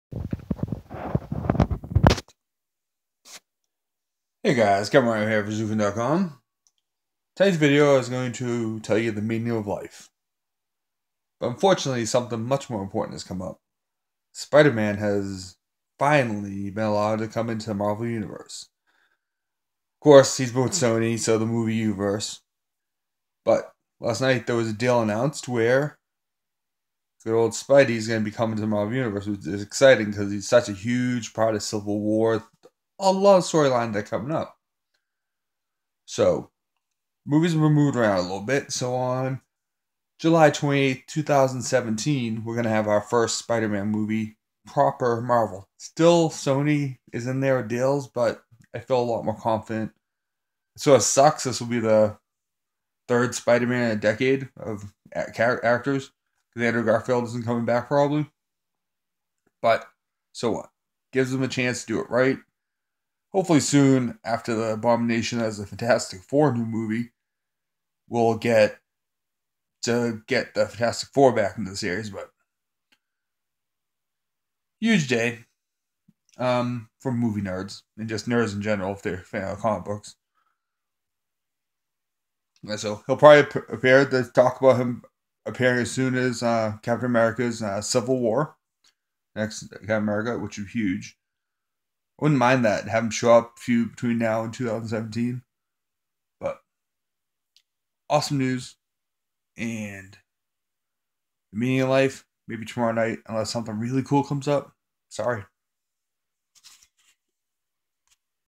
Spider-Man has come home to marvel
Finally Spiderman has come home to the Marvel cinematic universe